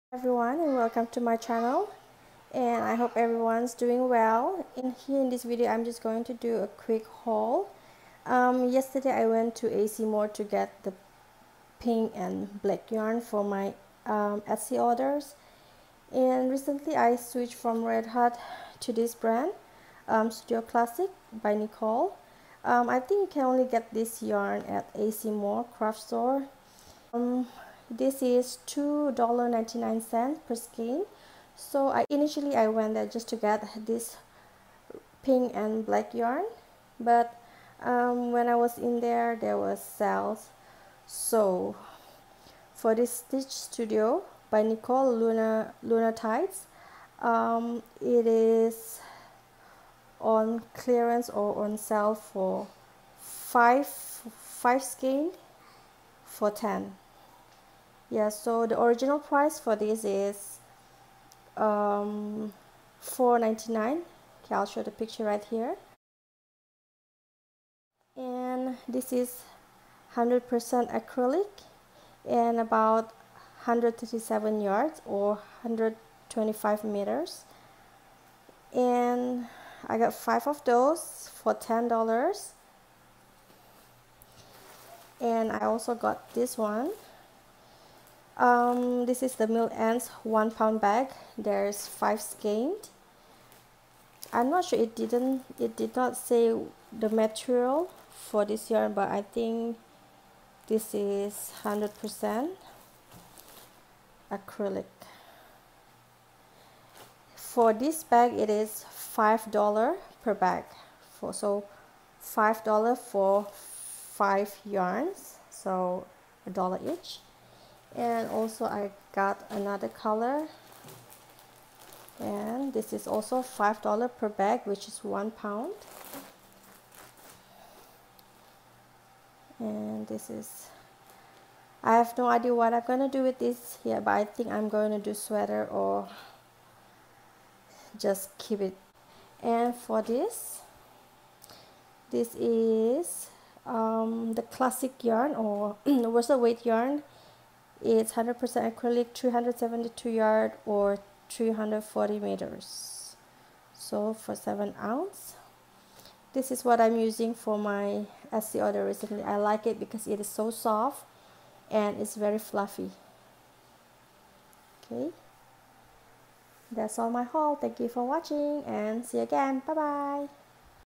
Yarn Haul - AC Moore craft store
Super happy with my purchase from AC Moore craft store. I can't resist great deal 🤗